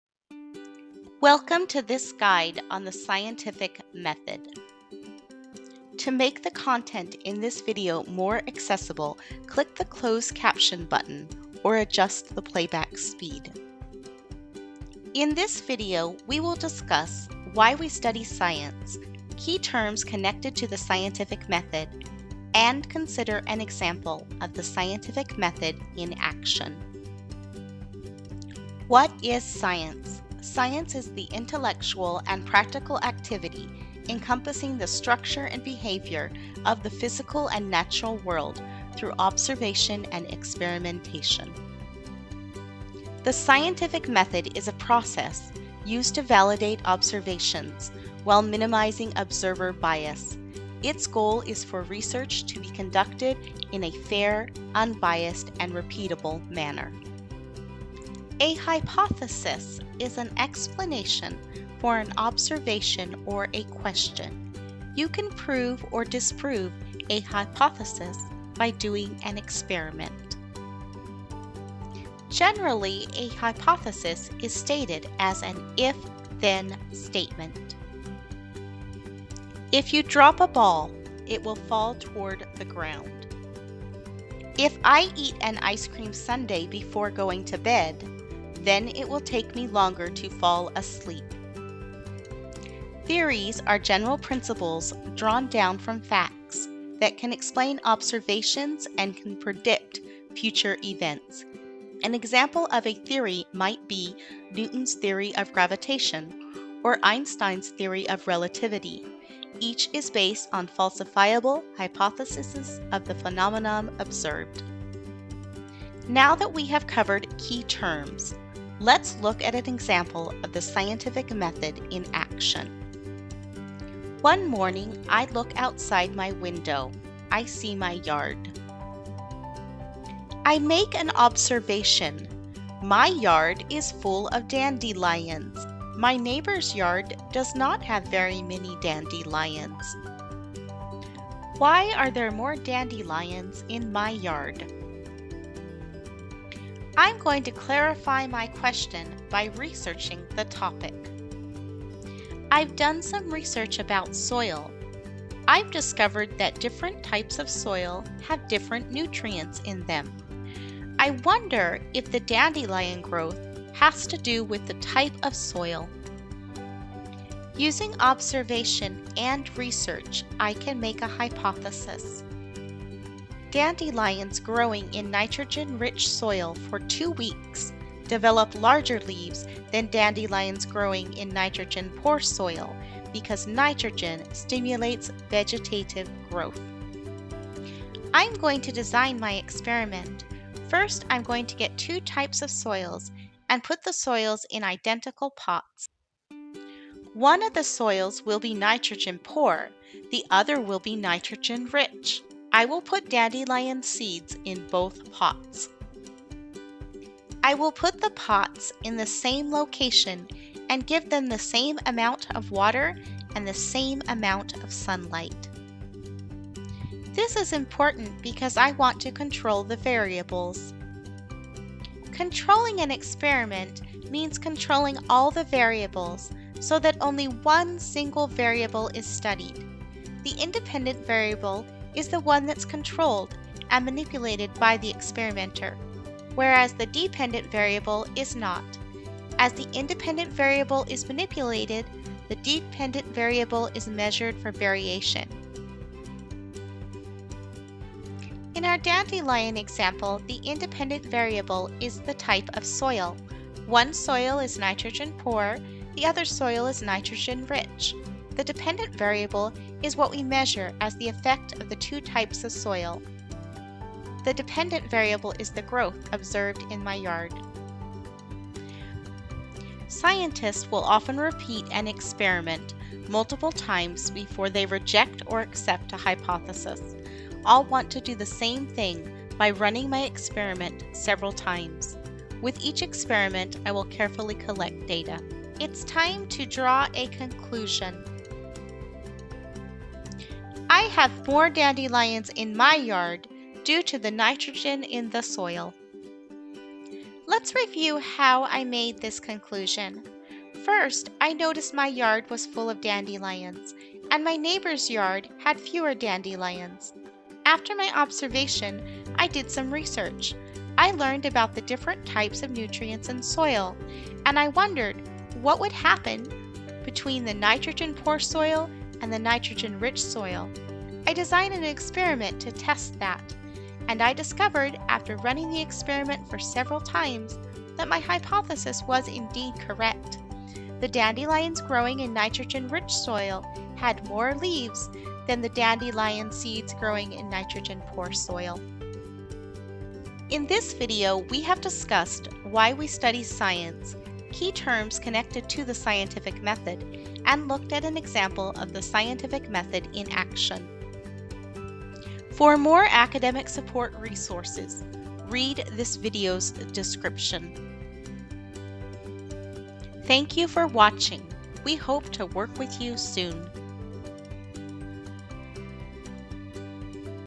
The Scientific Method
This video is a helpful basic overview of the Scientific Method.